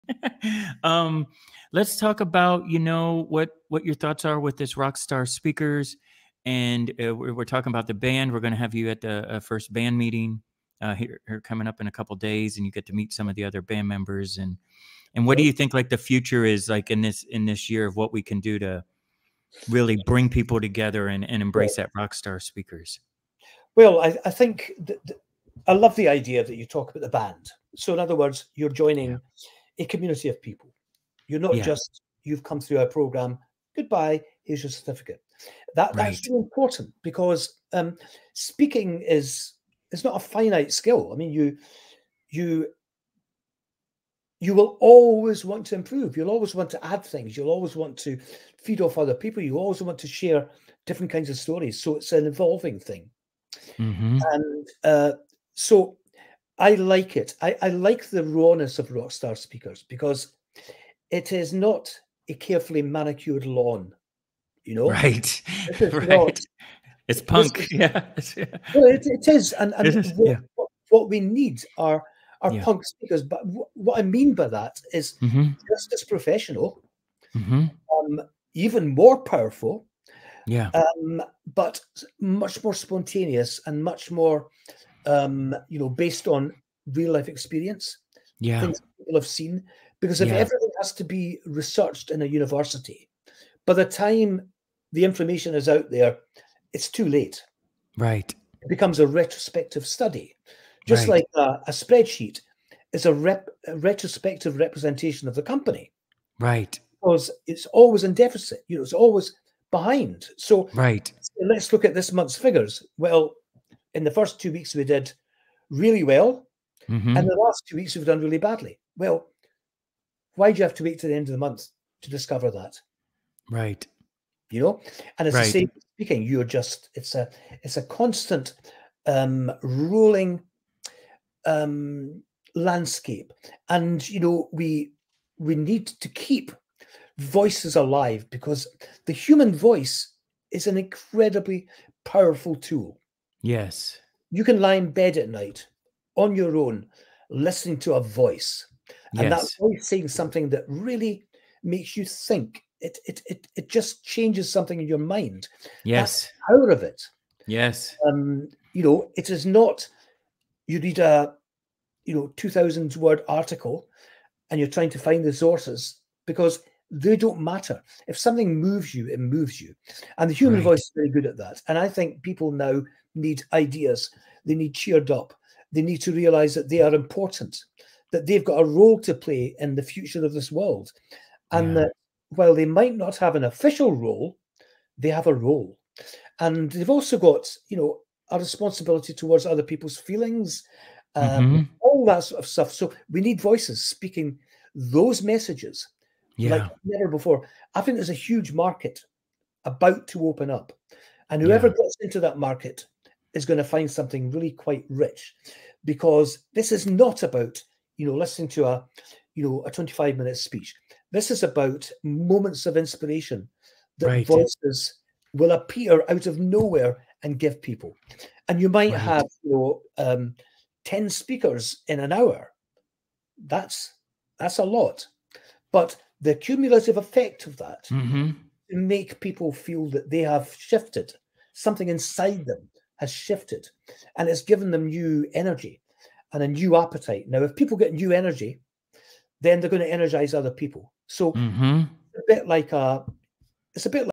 What is Rock Star Speakers? G's Backstage Pass with Mike Stevenson
What does it mean to be a Rock Star Speaker?

In this episode of G’s Backstage Pass I sit down with Mike Stevenson & we talk about what Rock Star Speakers is, & what he feels is the future of public speaking.

Enjoy this clip of our conversation. To hear the full episode check out G’s Backstage Pass

Rock Star Speakers is more than media training, it’s more than coaching.

Rock Star Speakers is a movement, it’s a mindset, & it’s the manifestation of your inner Rock Star.